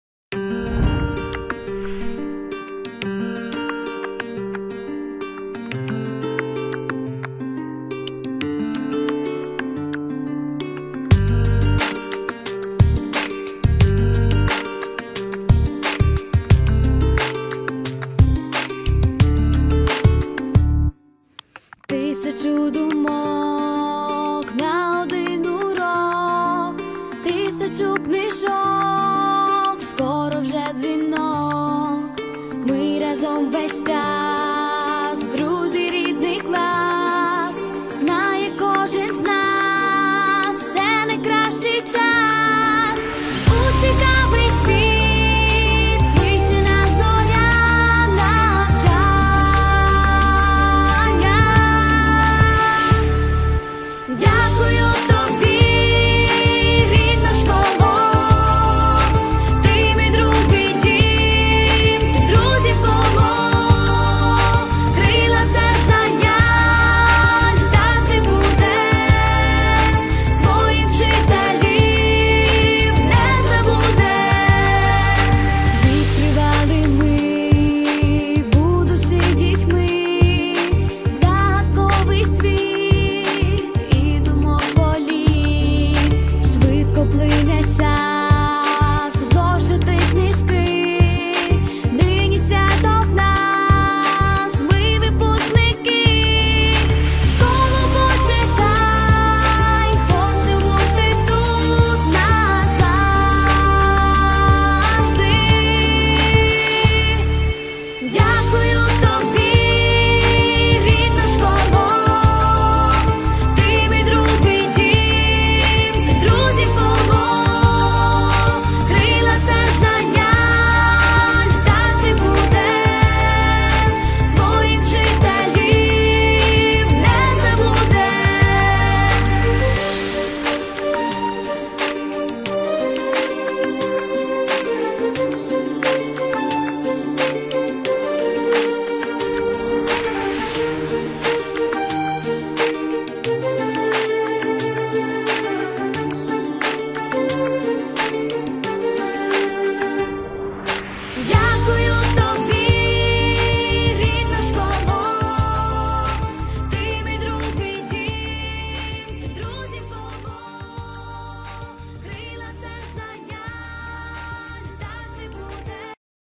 У Веренчанській ЗОШ відбувся «Вечір зустрічі випускників»
Випускники раділи зустрічам та охоче згадували кумедні миті які назавжди залишилися в пам’яті тих вже дорослих людей які давно закінчили школу та не забули до неї дорогу